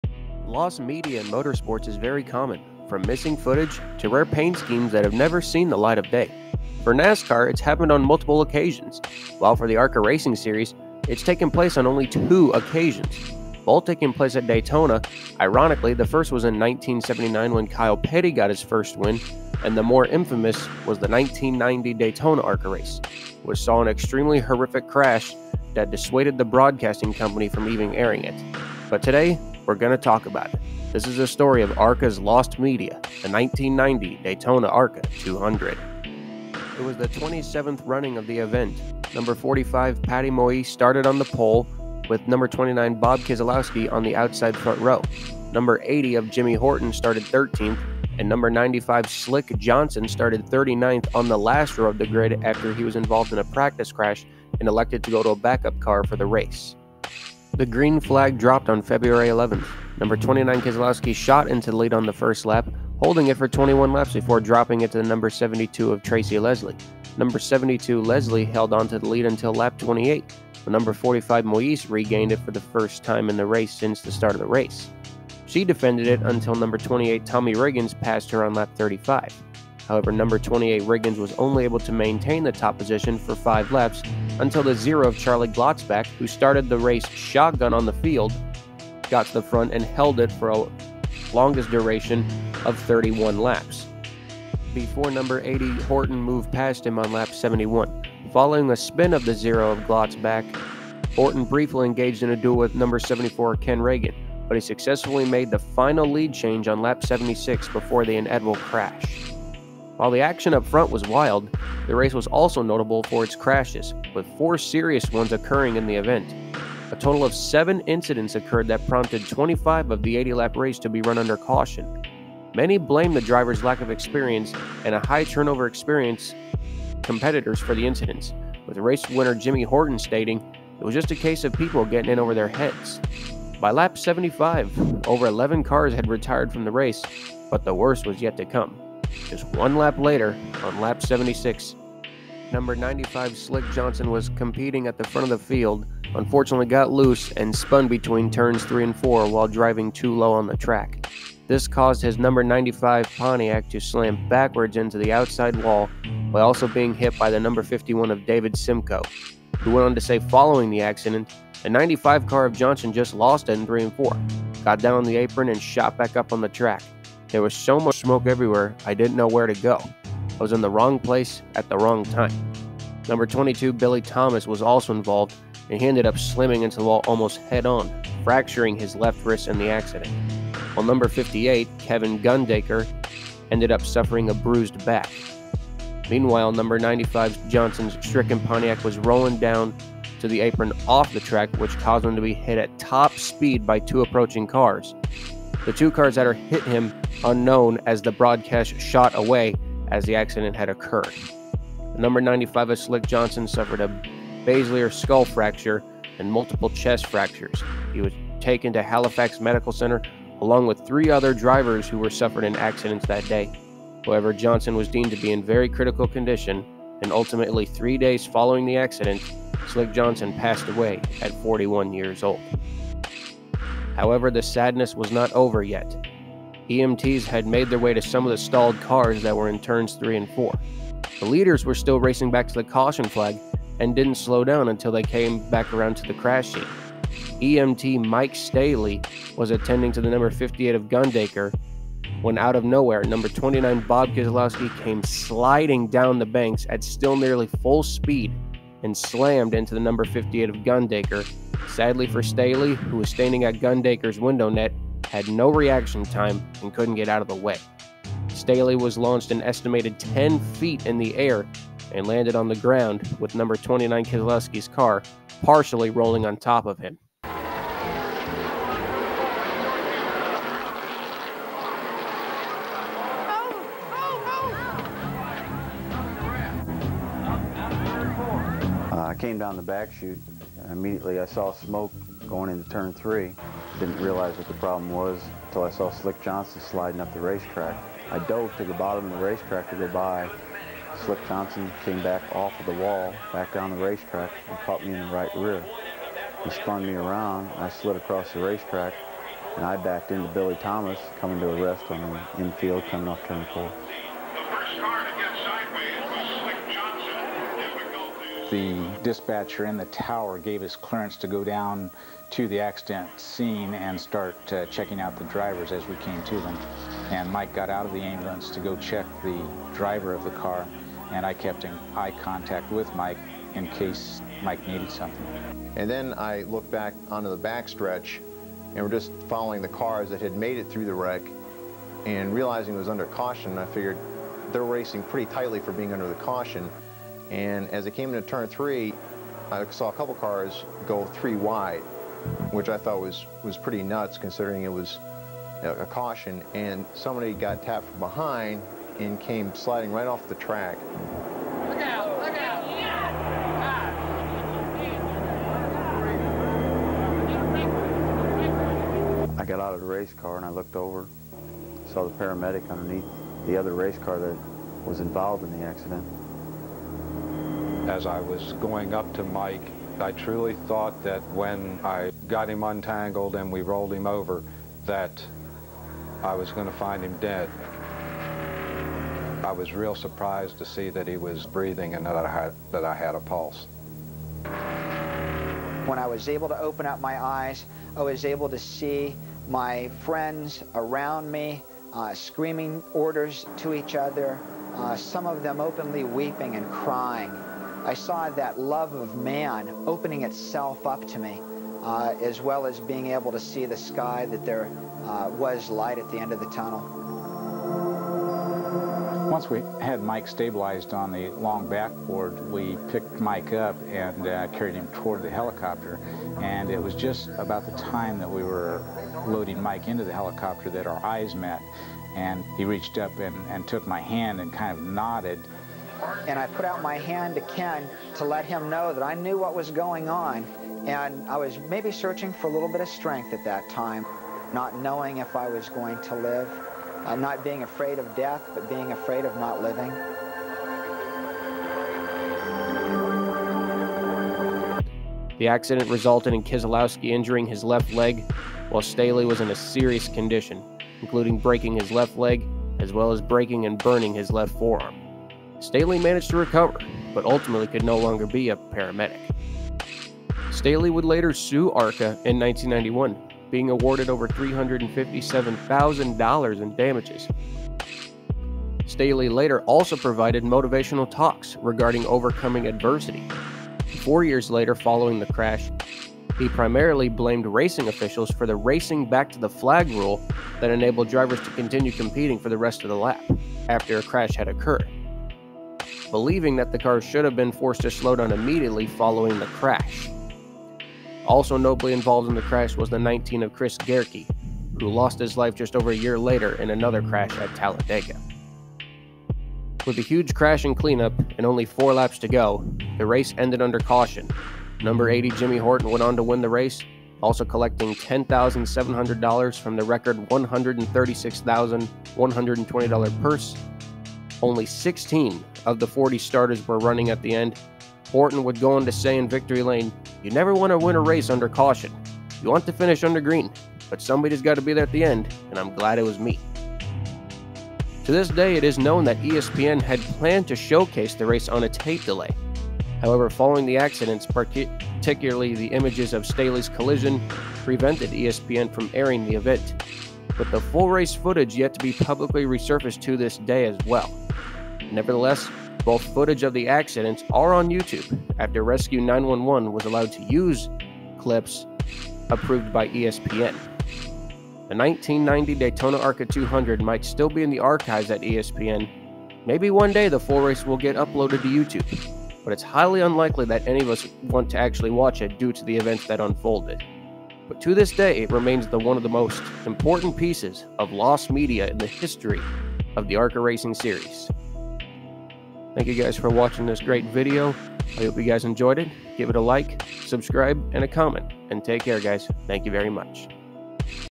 ARCA's Lost Media: The 1990 Daytona ARCA 200
The 1990 Daytona ARCA 200 is one of the most highlighted lost media in Racing history, from a crash that took the life of a driver to an EMT getting hit on Track as well

The race was scheduled to air on a tape delay on ESPN, but to this day over 32 years later, it has yet to be shown, only the crash itself is on here. I hope everyone enjoy's this Doc.

I'd like to thank the following Pics: